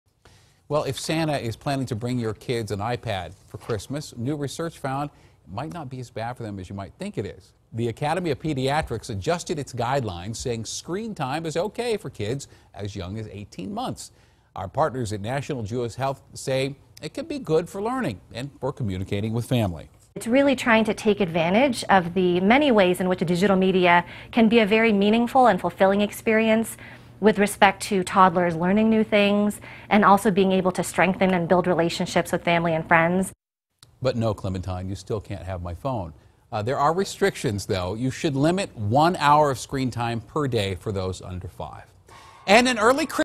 New Guidelines For Your Kids' Screen Time
The Academy of Pediatrics revised its guidelines for screen time for kids over 18 months. Dr. Carah Santos, pediatric allergist at National Jewish Health in Denver, explains how digital media can be beneficial.